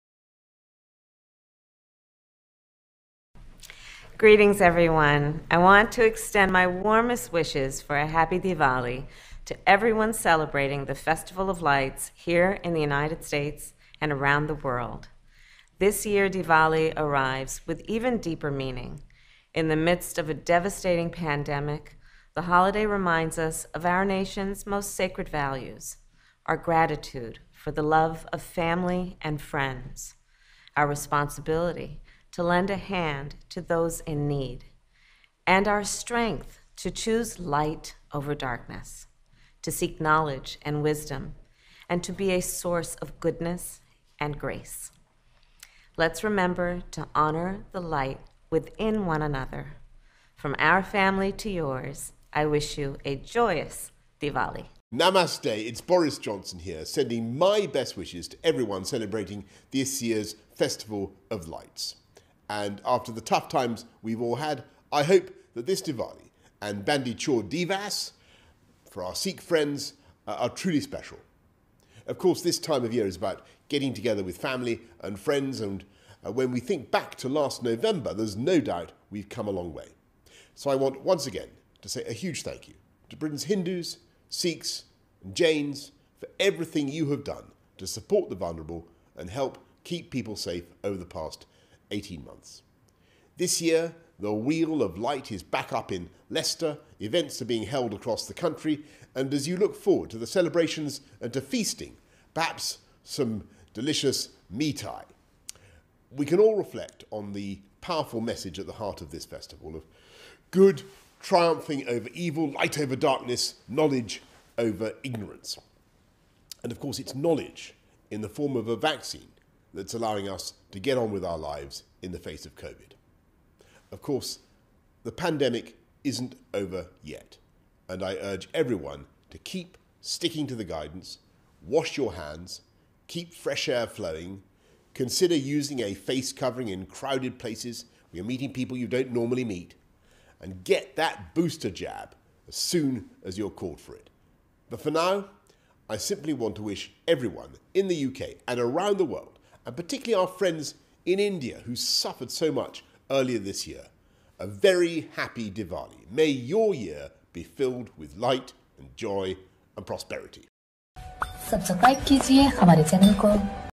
US VP Kamala Harris | UK PM Boris Johnson - Worldnews24
New York, the US । For the first time ever, Diwali-themed animation adorned One World Trade Center. Fireworks were also displayed that was viewed by audiences on both sides of the Hudson River.
Happy Diwali to everyone celebrating the festival of lights in the US & around the world. This yr Diwali arrives with an even deeper meaning in midst of a devastating pandemic. The holiday reminds us of our nation's most sacred values । US VP Kamala Harris
US VP Kamala Harris
(Source: US VP's Twitter)

After the tough times we've all had, I hope that this Diwali & Bandi Chhor Divas are truly special. This time of yr is about getting together with family & friends. When we think back to last Nov there's no doubt we've come a long way । UK PM Boris Johnson
UK PM Boris Johnson